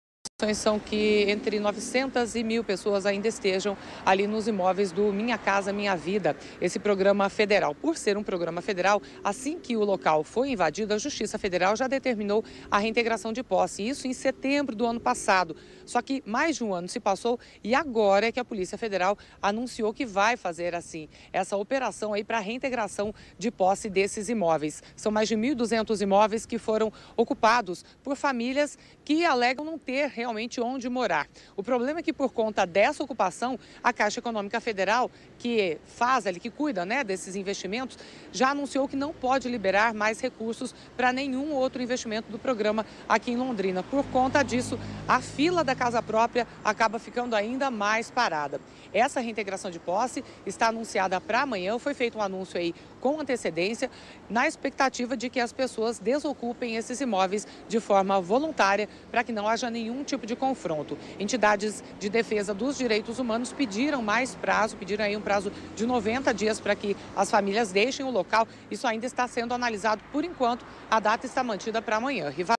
PF anuncia retirada 900 pessoas do residencial Flores do Campo em Londrina
Assim que o local foi invadido, em setembro de 2016, a Justiça Federal já determinou a reintegração de posse. Após um ano, a Polícia Federal anunciou a operação parar tirar as famílias do local. Confira imagens!